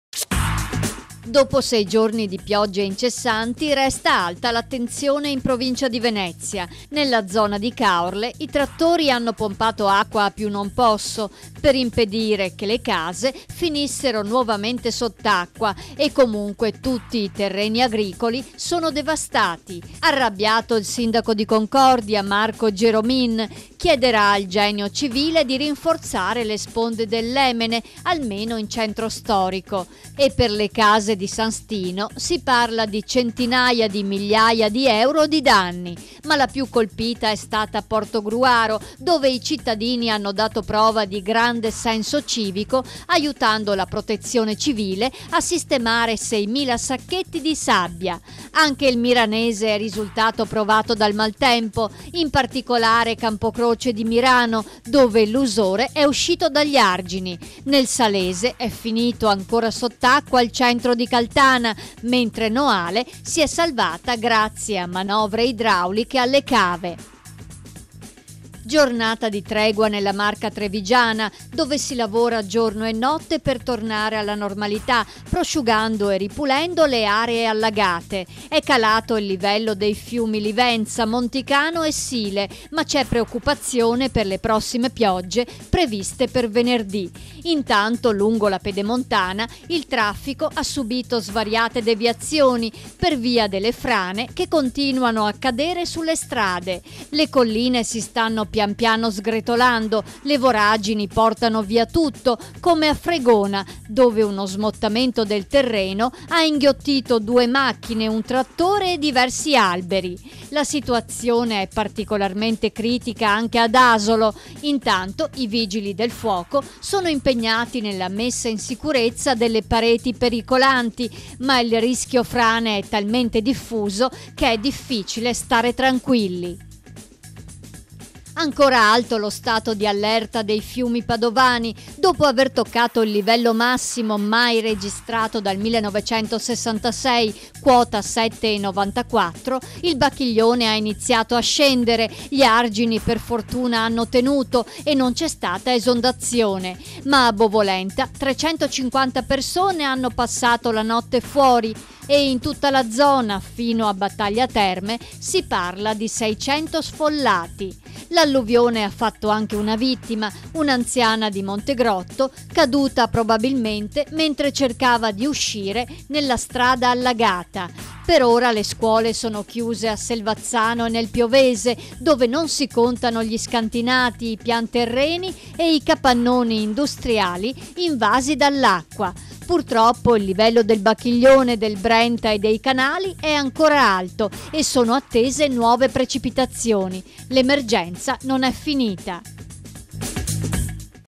Le brevi del 05 Febbraio 2014
VENEZIA - Maltempo, la situazione in provincia
TREVISO - Tregua del maltempo, continua il rischio frane
PADOVA - Allagamenti: centinaia di sfollati